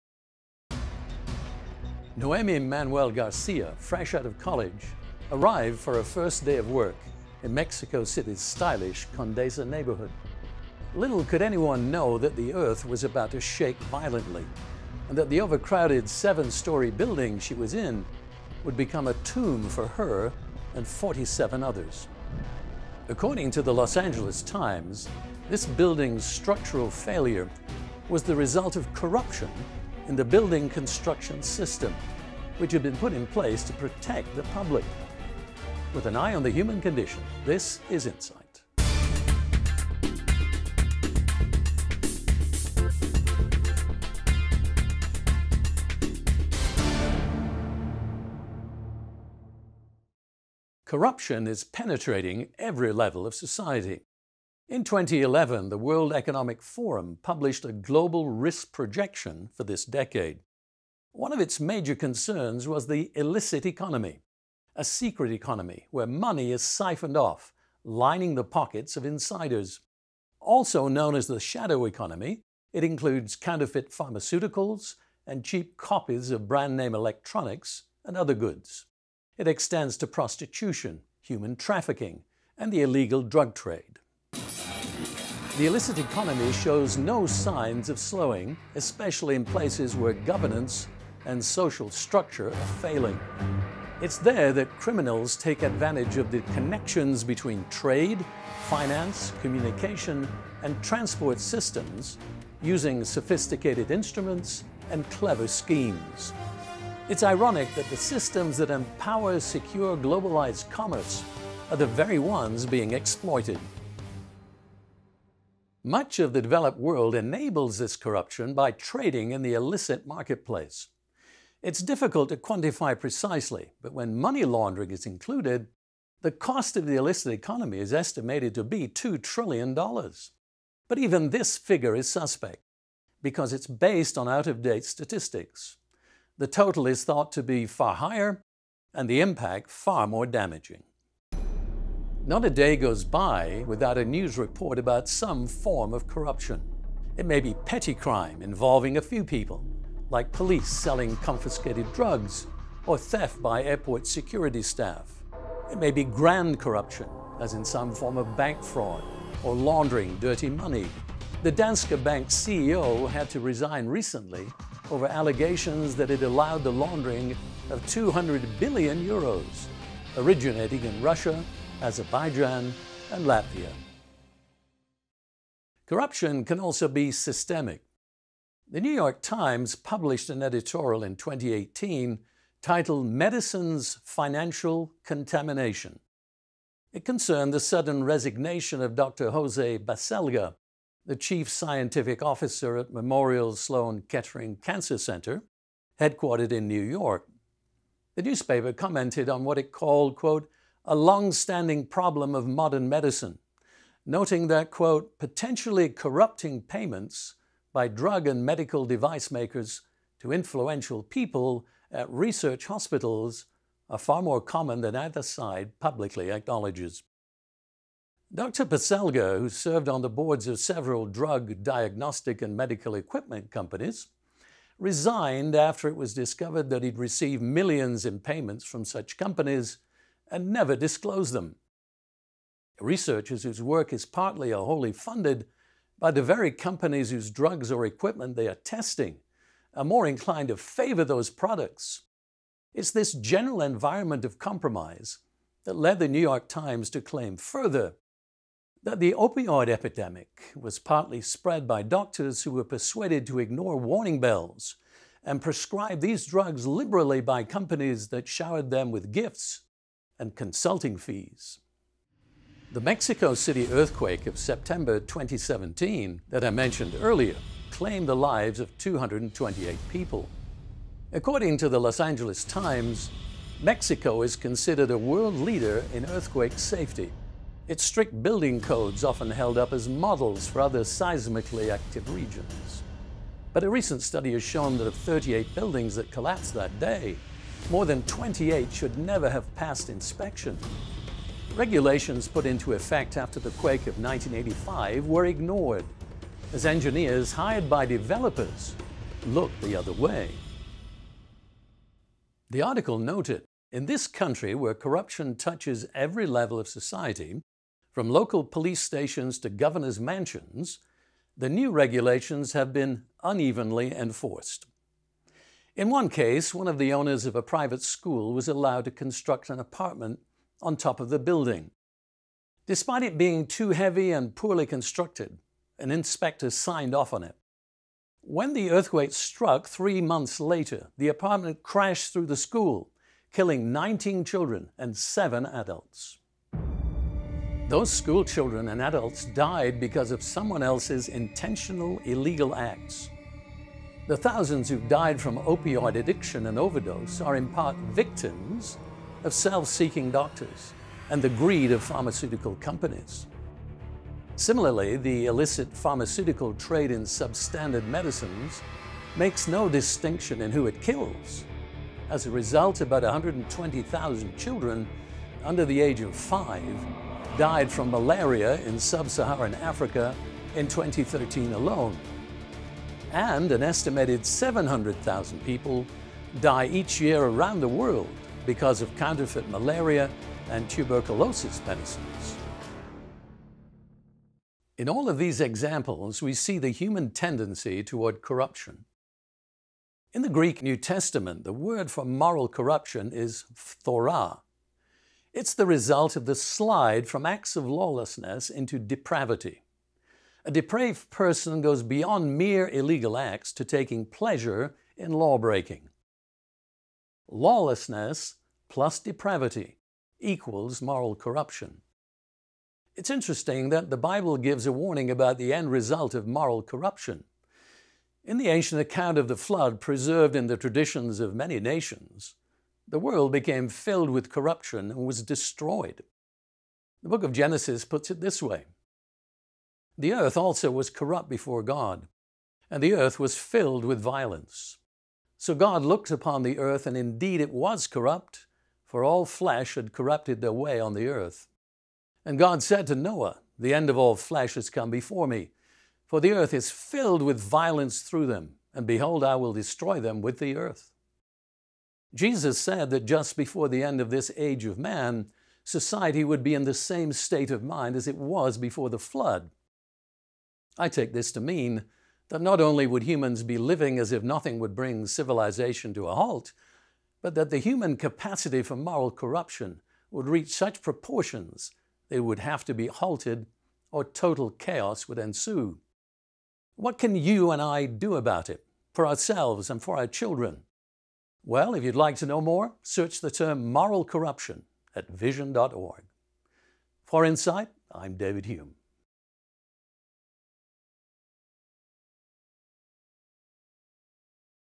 The Curse of Corruption | Insight with David Hulme
Corruption is penetrating every level of society. In 2011, the World Economic Forum published a global risk projection for this decade. One of its major concerns was the "illicit economy"—a secret economy where money is siphoned off, lining the pockets of insiders. Also known as the "shadow economy," it includes counterfeit pharmaceuticals, cheap copies of brand-name electronics and other goods. It extends to prostitution, human trafficking and the illegal drug trade. The illicit economy shows no signs of slowing, especially in places where governance and social structure are failing. Much of the developed world enables this corruption by trading in the illicit marketplace. What are the causes of this level of worldwide exploitation?